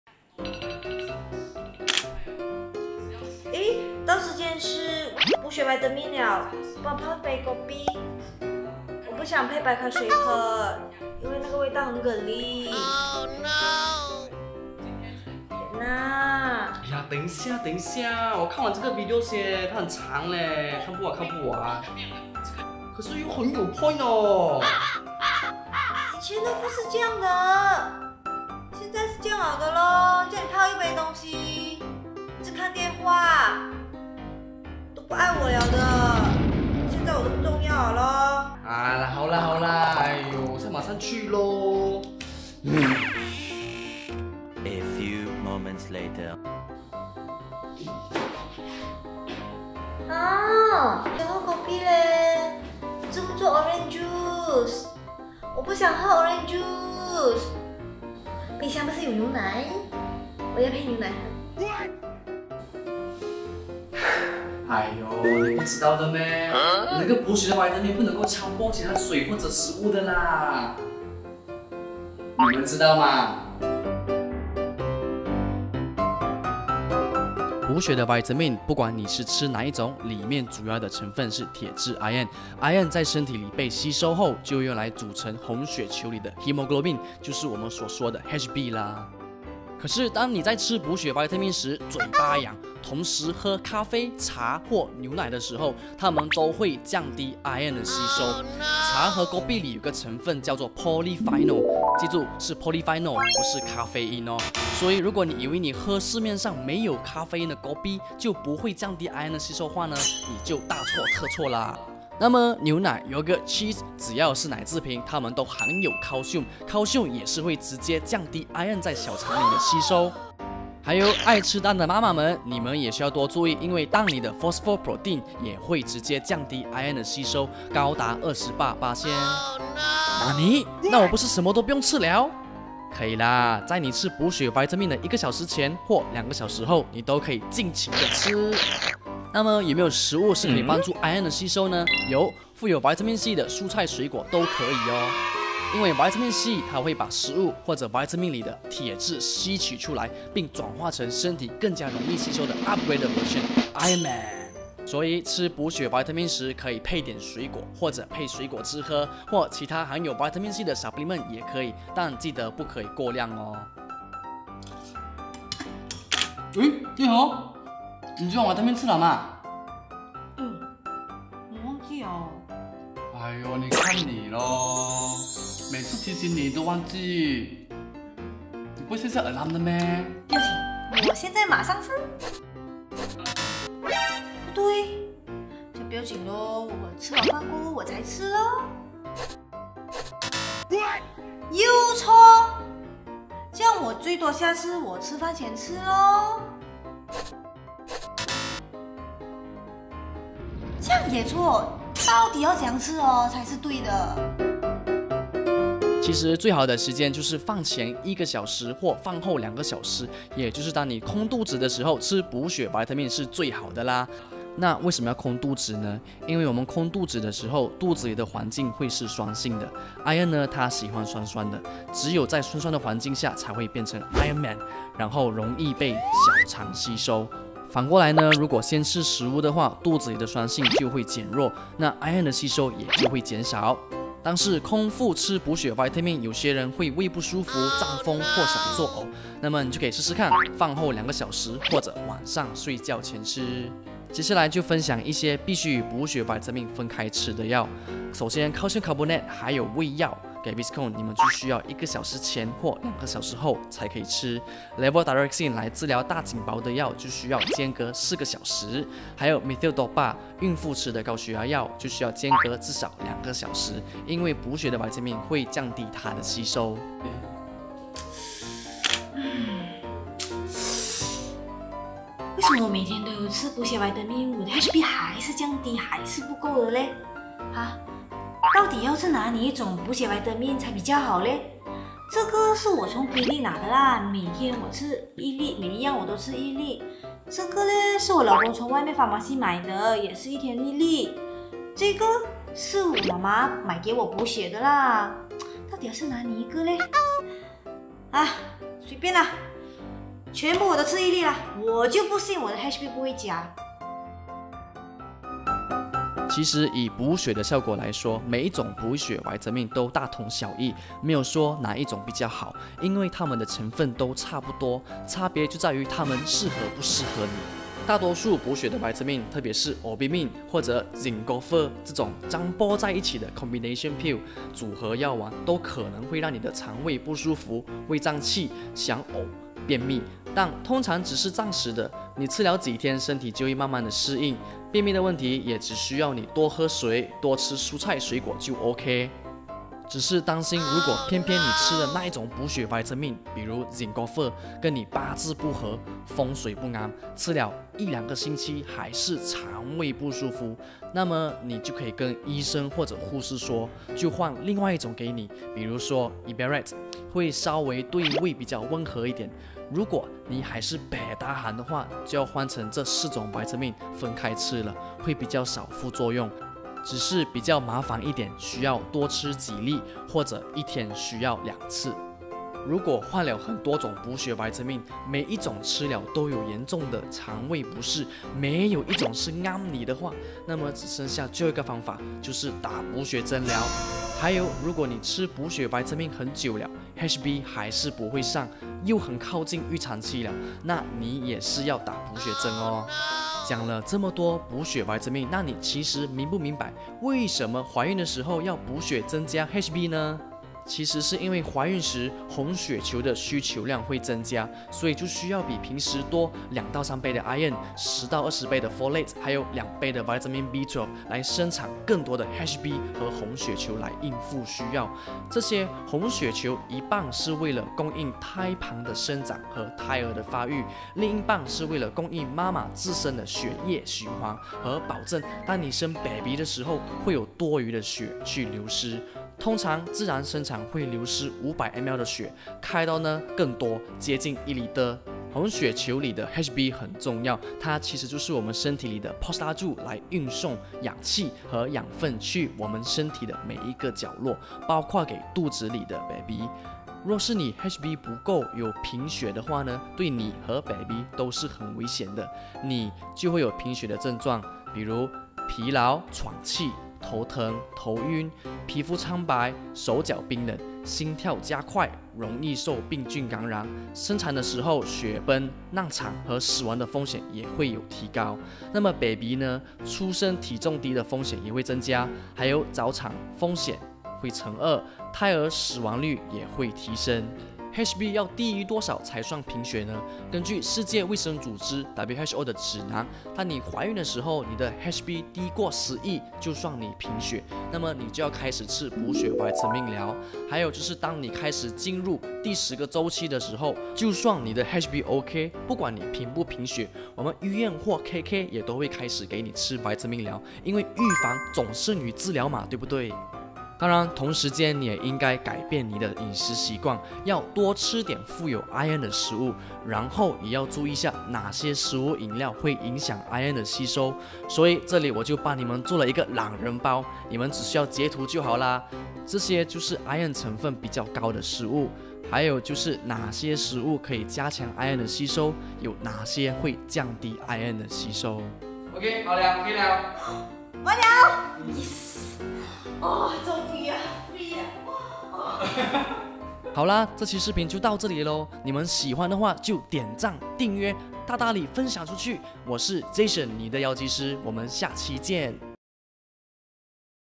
怀孕贫血该吃什么维生素？补血维生素正确吃法 • 补血营养食物 【vitamin for pregnancy • anemia during pregnancy • iron supplements】
怀孕时拼命吃补血维他命 (铁质保健品），但为什么红血球蛋白 (Hb) 还是这么低，不会增加呢？还是贫血呢？为什么怀孕时会贫血呢？有没有想过是什么原因呢？看了这个视频，也许你就会找到你要的答案啦！

免责声明：
我在视频中所提到的药物仅用于教育目的。

谢谢🙏🏻

Why I got anemia during pregnancy? Why my blood (hemoglobin) still at low level no matter how much i ate the iron supplements (vitamin for blood)? Ever wonder what are the reasons behind? Watch this video and find out yourself! ^^